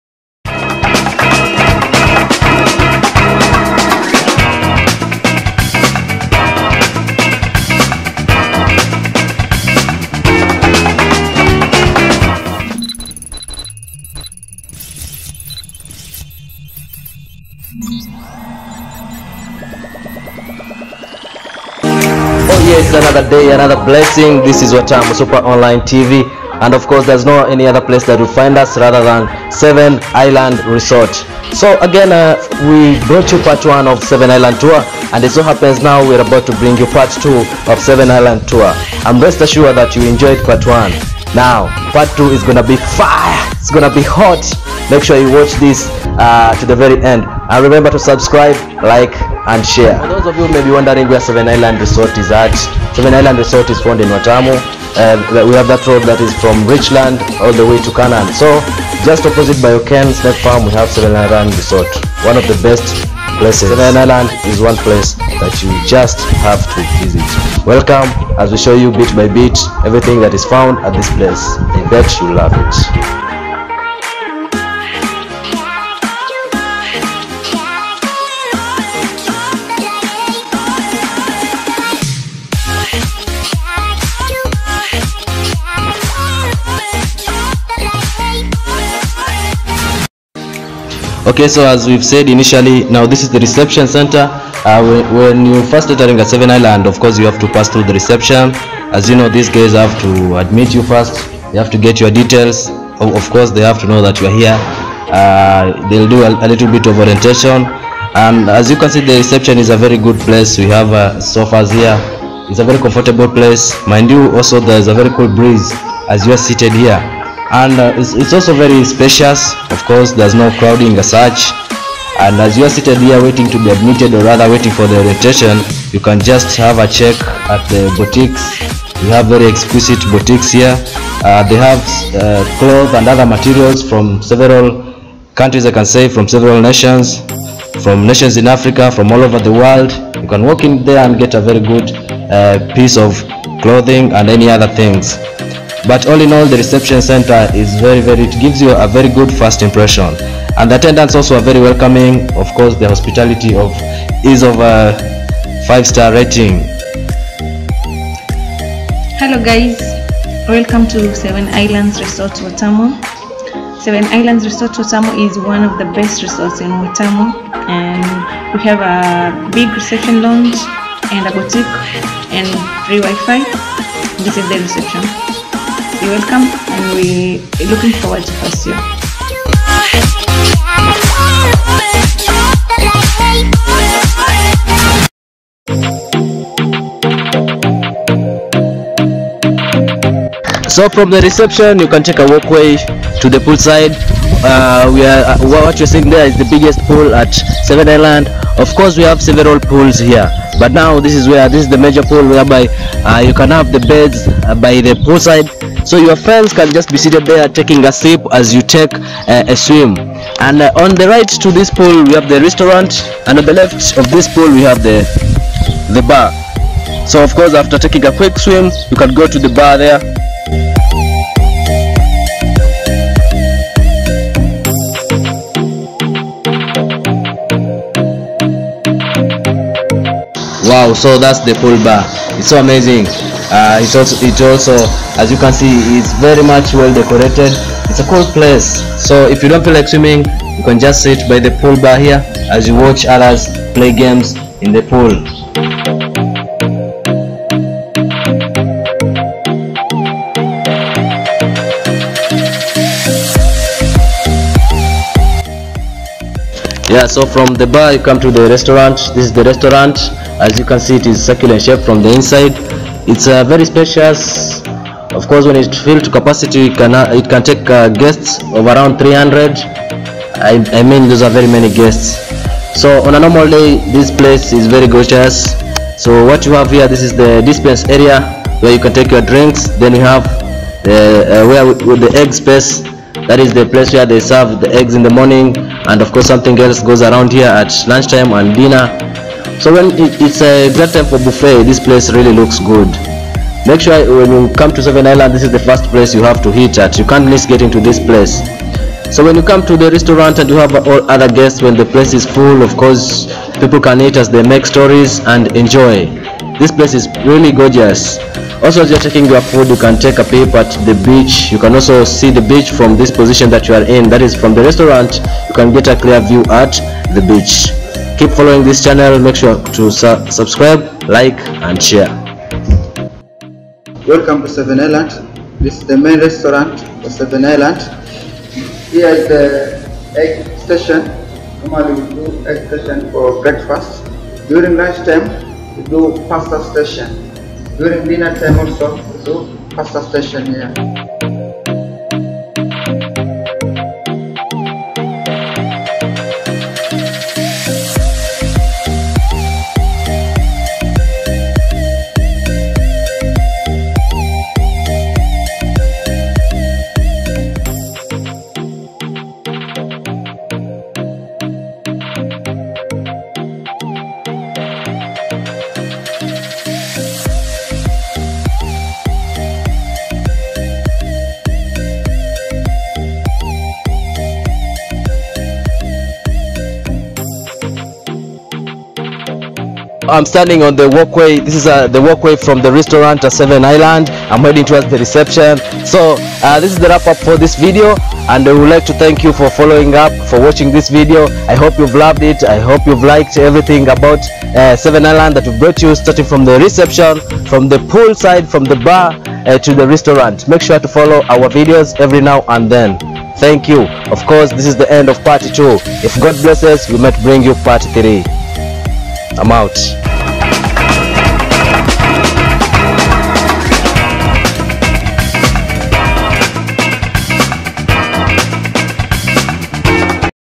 Part2:SEVEN ISLAND RESORT THE VACATION PARADISE!
Seven island resort is definitely any tourists dream destination. The place is just magnificent boosting of exquisite sites and marvelous hospitality. Your travel journal isn't complete without visiting Seen island resort.